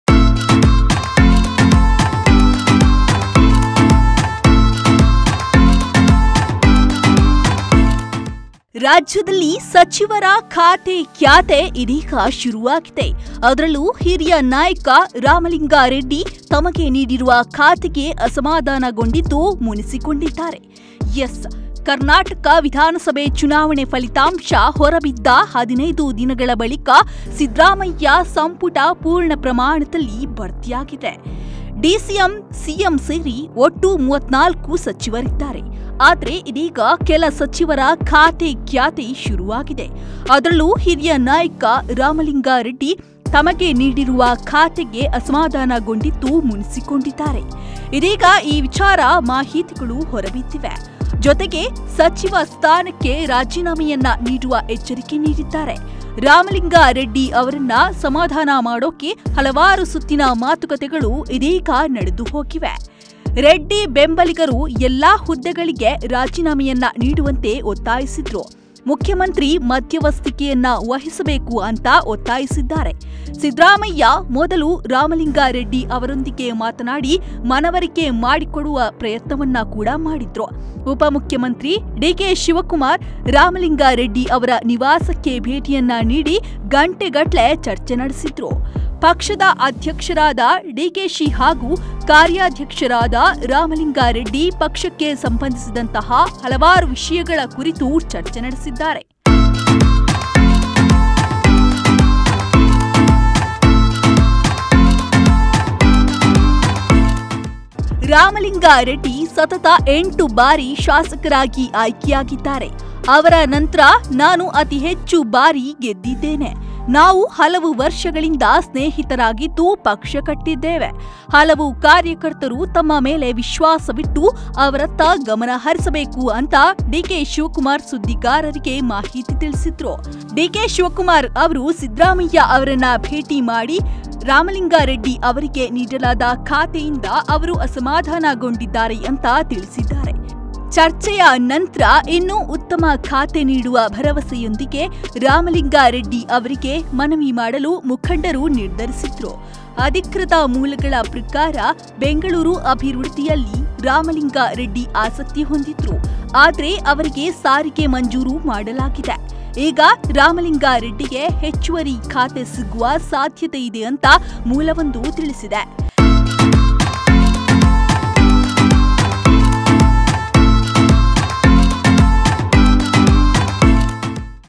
KSRTC : ಸಂಕಷ್ಟದಲ್ಲಿರುವ ಸಾರಿಗೆ ಬೇಡವೆ ಬೇಡ | ಮನವೊಲಿಸಲು ಸಿಎಂ-ಡಿಸಿಎಂ ಸುಸ್ತೋ ಸುಸ್ತು !
KSRTC : ramalingareddy : ಸಂಕಷ್ಟದಲ್ಲಿರುವ ಸಾರಿಗೆ ಬೇಡವೆ ಬೇಡ | ಮನವೊಲಿಸಲು ಸಿಎಂ-ಡಿಸಿಎಂ ಸುಸ್ತೋ ಸುಸ್ತು !